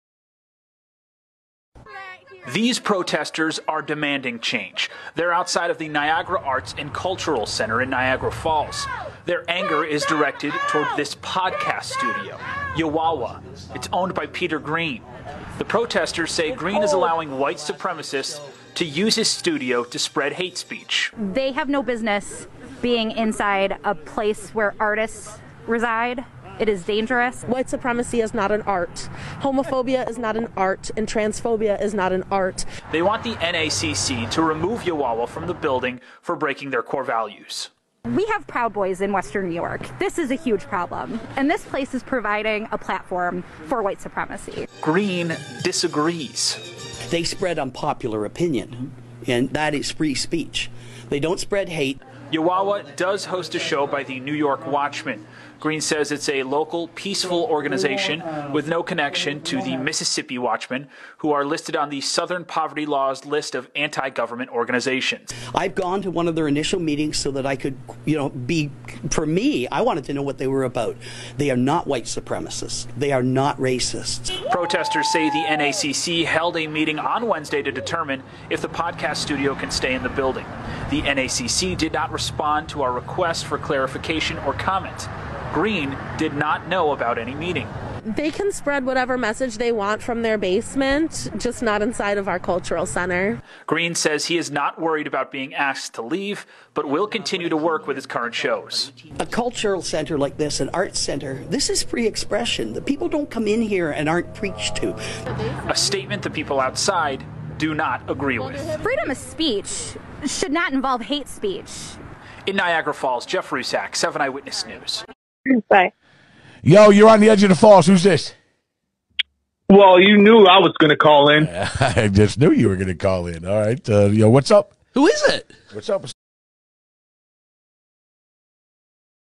HATE HAS NO HOME HERE
when you are racist, xenophobic, homophobic, transphobic, sexist, and a pedophile sympathizer, comparing being arrested for child porn to shoplifting, I’m sorry but you gotta go. we work so hard to lift up our community, we will not allow this type of rhetoric to be broadcast representing the people of buffalo and Niagara Falls. i am pro free speech but not pro hate speech and free speech ≠ free from consequences. the board agreed and as of 2/28 they will be removed. i support disagreement, i support argument, i support you even if i don’t agree with you, but i don’t support harassment of domestic violence survivors, sexual abuse survivors, and doxxing people who you don’t agree with. i am so grateful to the board at NACC for taking real action and removing the hate speech from a community art center, where artists are afraid to go because of this podcast. all should be welcome and feel safe.